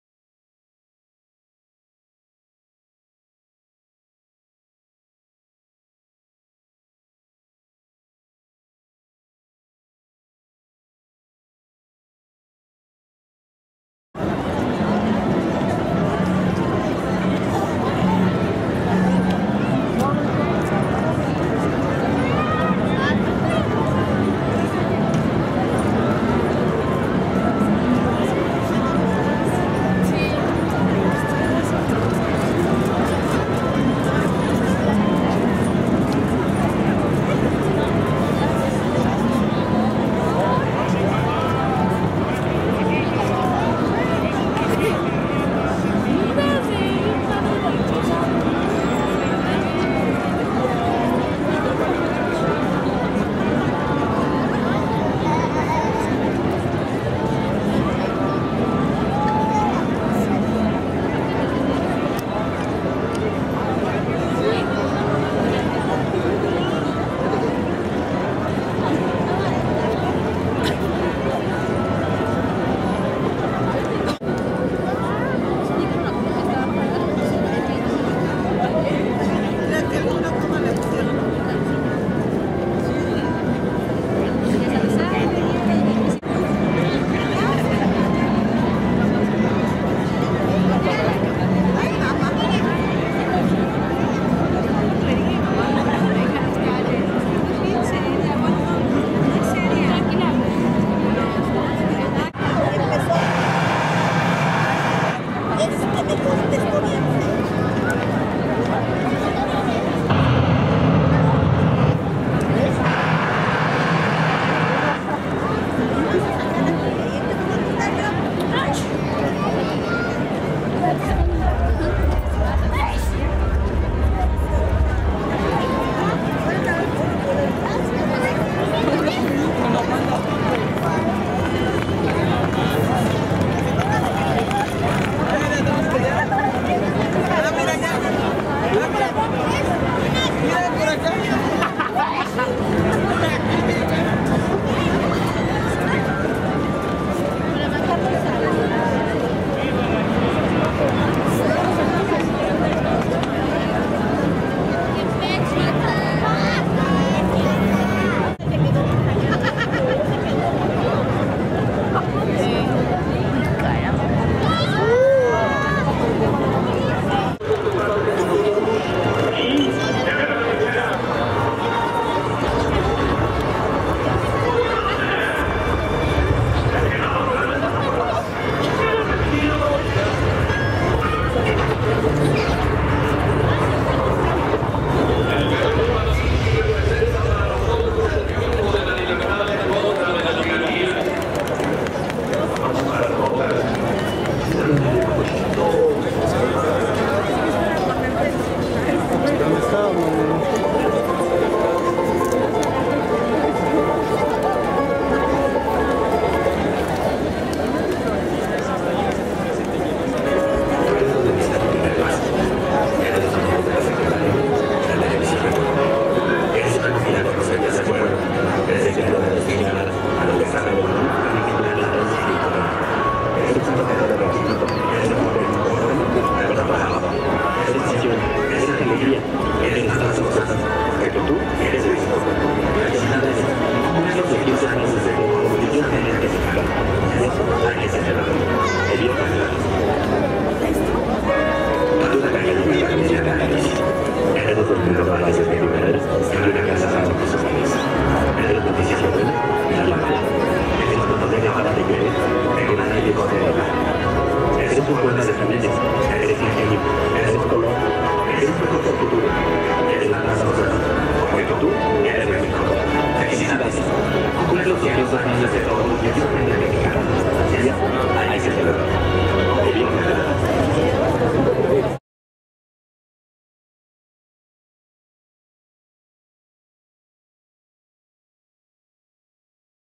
Previo al espectáculo - Yo México
Una breve mirada a la expectación antes del inicio de este excelente espectáculo  - Yo México - el cual pudimos disfrutar miles de personas.